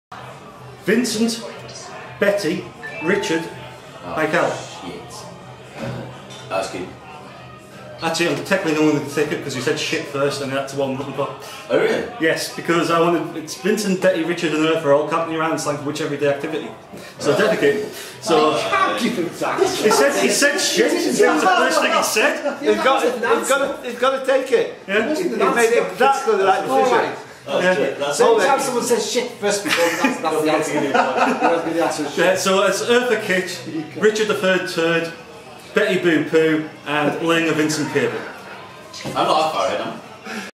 LightSpeed Quiz: Perfect call by the question master
LightSpeed Quiz 2017 Tournament Seven from Thornaby-on-Tees

You don't have to agree with me, but this is one of those rare cases when ... well just have a look

Set and read by Chris Bean

Kudos

To join in, type "LightSpeed Quiz" into facebook
OR TRY